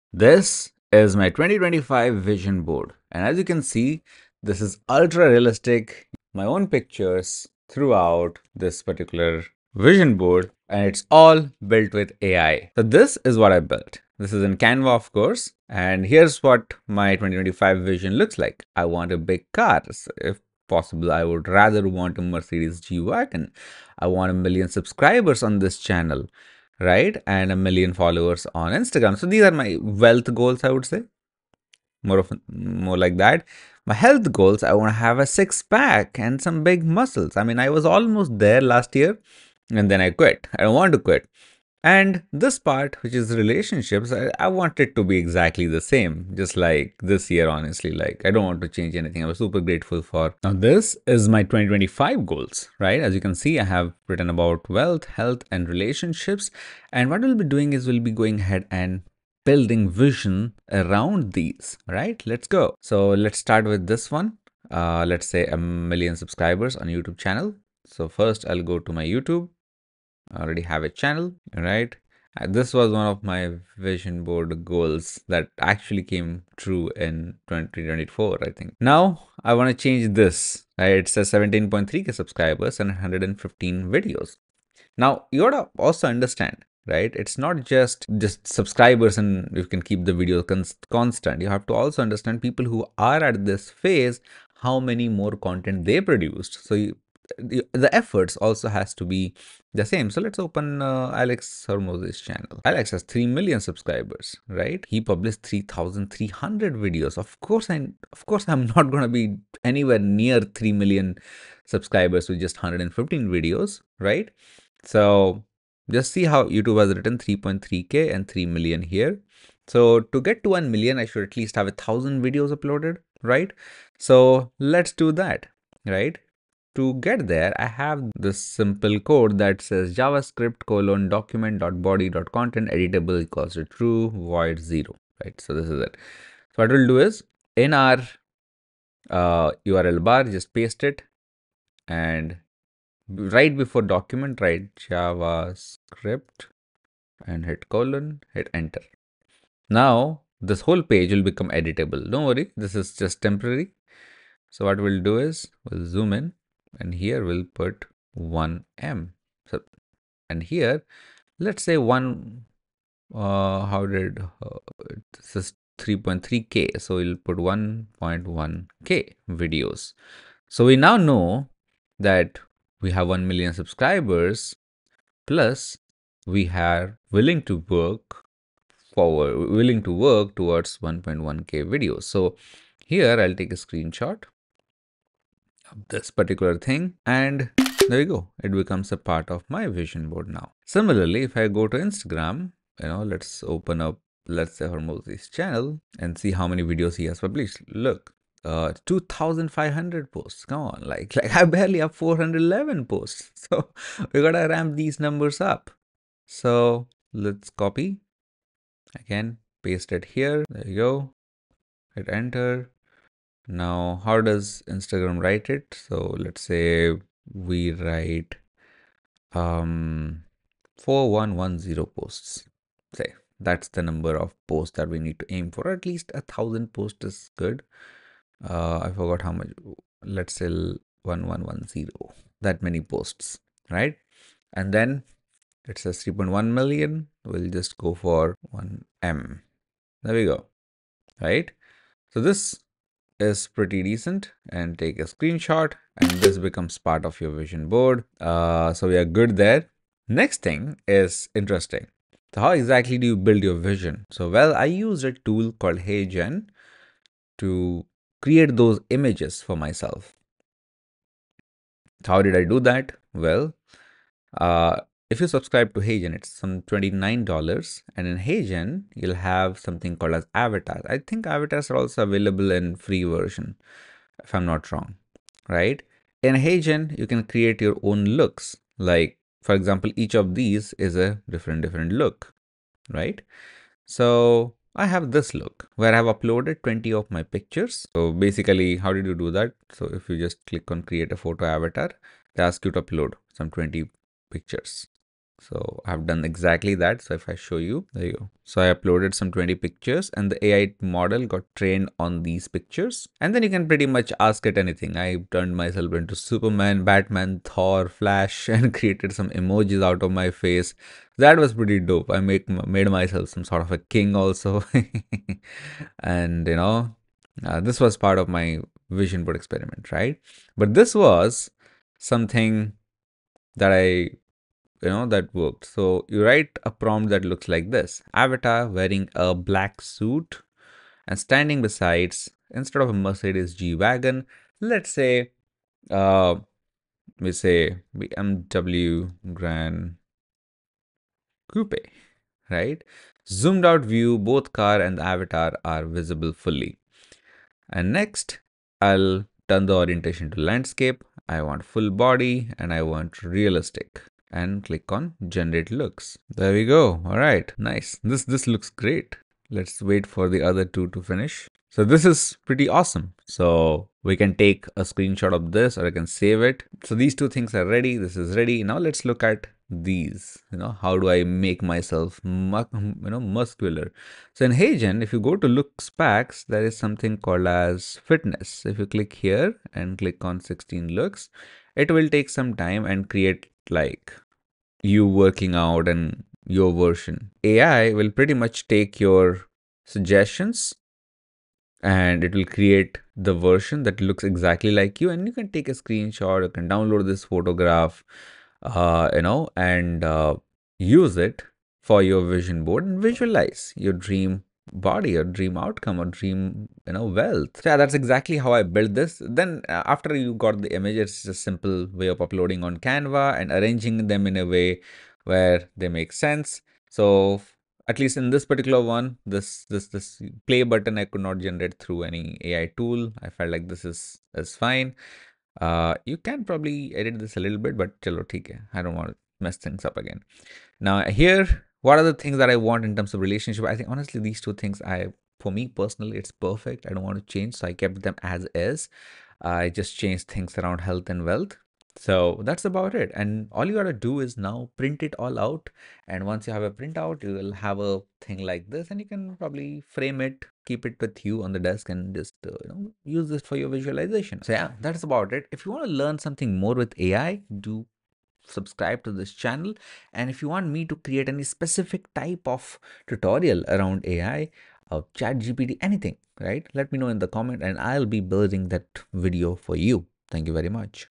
How to Create a Realistic Vision Board with AI
. board using the power of AI and tools like Canva and ChatGPT. This isn’t just any vision board – it’s ultra-realistic, personalized, and designed to keep you motivated toward your goals.

Here’s what you’ll learn:

How to make a vision board that truly reflects your dreams in areas like wealth, health, and relationships.
Step-by-step instructions for using AI tools to create custom images for your board.
How to use ChatGPT and Canva to organize and design your vision board for maximum impact.
Tips to create a vision board with measurable goals, like achieving 1 million YouTube subscribers or fitness transformations.
Whether you’re visualizing success in your career, personal life, or fitness journey, this guide will help you build a vision board that inspires action and delivers results.

Don’t forget to comment with your questions or ideas for future videos!